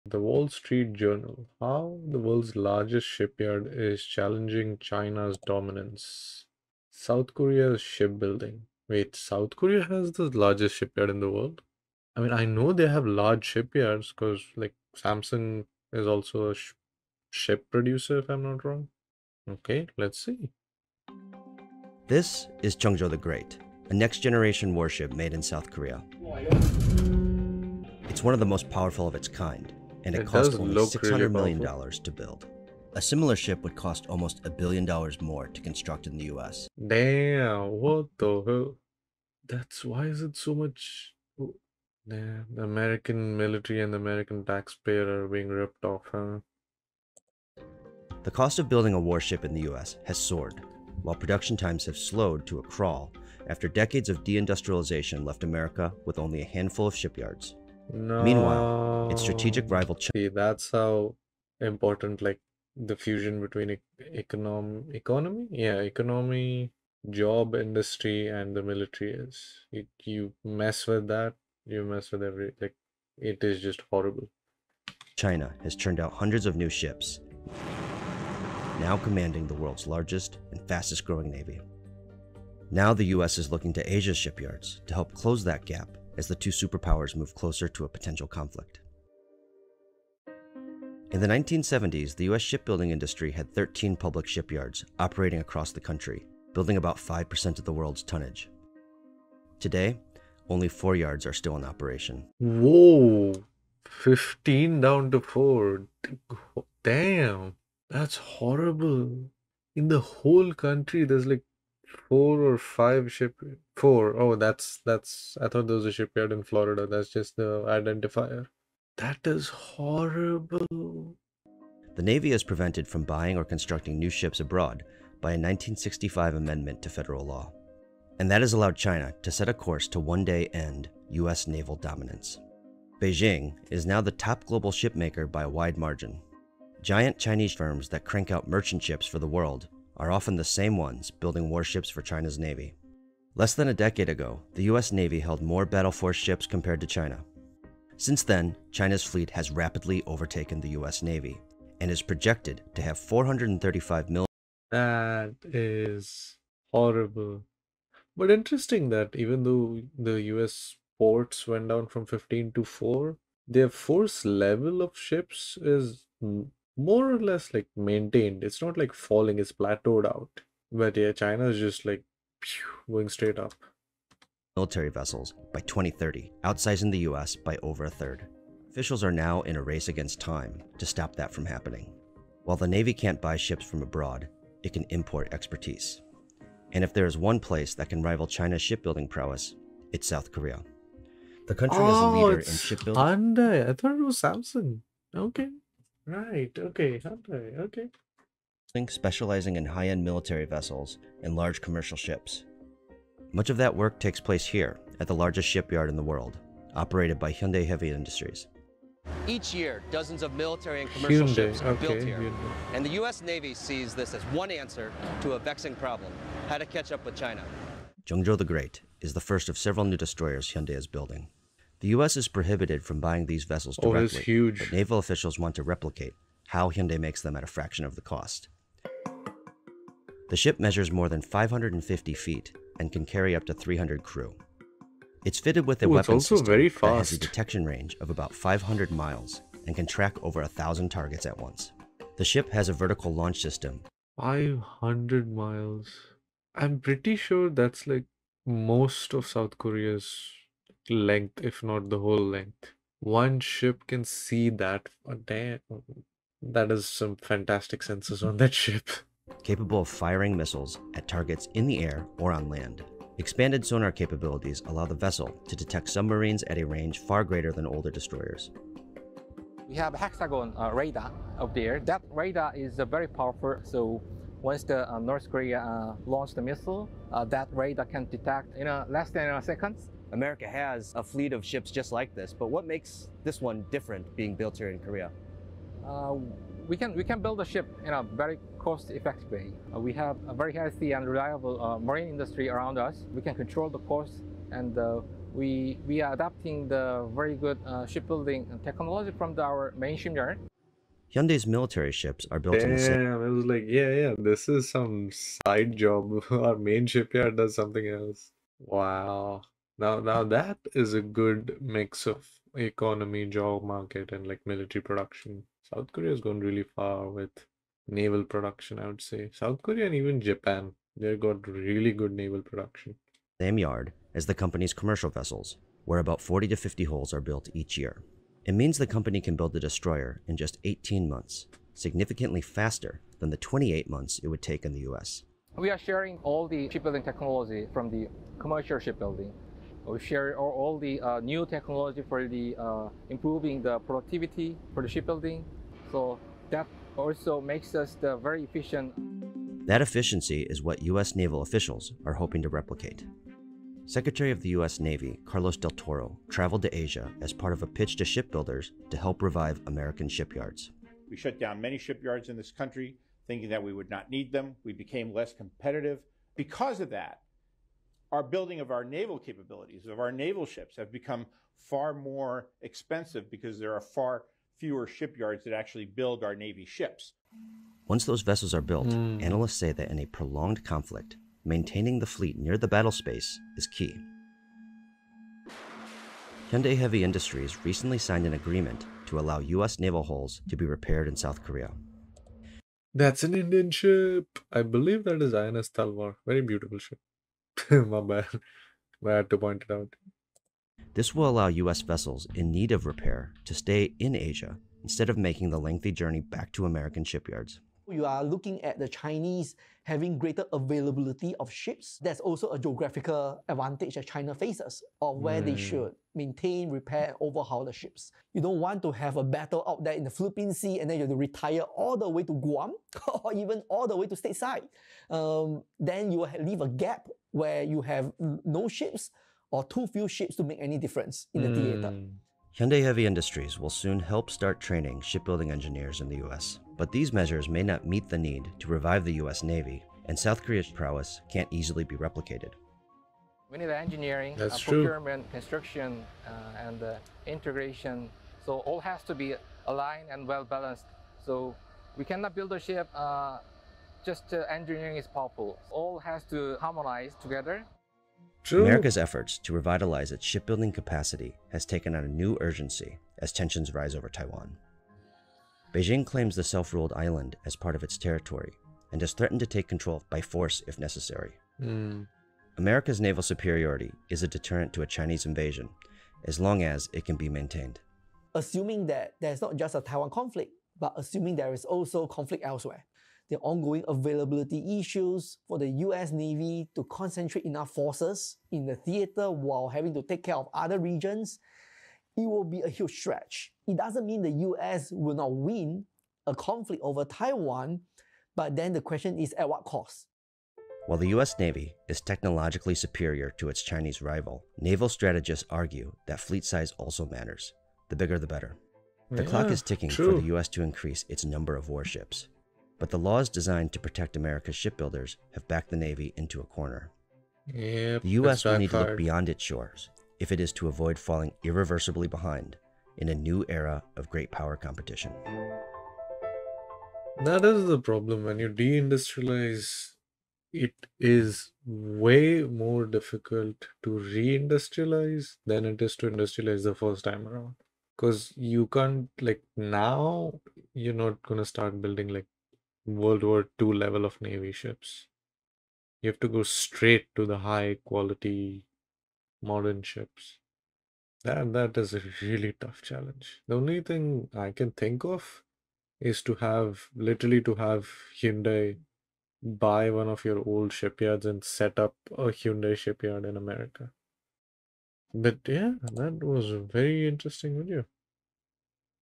WORLD'S BIGGEST SHIPYARD NOT AMERICAN!? Reacting to naval shipbuilding
In this recorded stream we take a look at India's Nuclear Submarine Base Development Program through the lens/ Video of a well known channel -